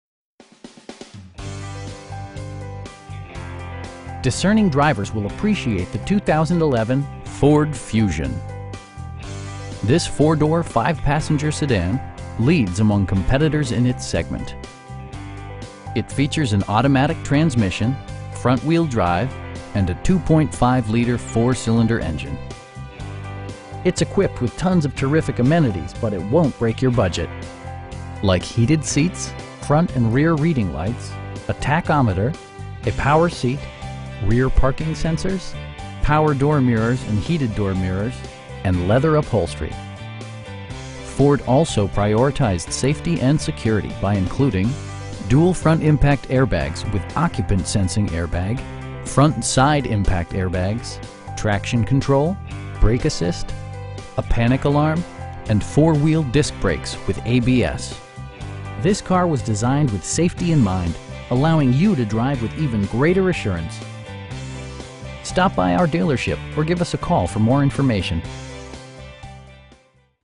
2011 Ford Fusion SEL in Buford, GA 30518
4525 Nelson Brogdon Blvd.

Discerning drivers will appreciate the 2011 Ford Fusion. This 4 door, 5 passenger sedan leads among competitors in its segment. It features an automatic transmission, front-wheel drive, and a 2.5 liter 4 cylinder engine. It's equipped with tons of terrific amenities, but it won't break your budget. Like heated seats, front and rear reading lights, a tachometer, a power seat, rear parking sensors, power door mirrors and heated door mirrors, power windows, and leather upholstery. Ford also prioritized safety and security by including: dual front impact airbags with occupant sensing airbag, front SIDE impact airbags, traction control, brake assist, a panic alarm, and 4 wheel disc brakes with ABS. This car was designed with safety in mind, allowing you to drive with even greater assurance. We pride ourselves in the quality that we offer on all of our vehicles. Stop by our dealership or give us a call for more information.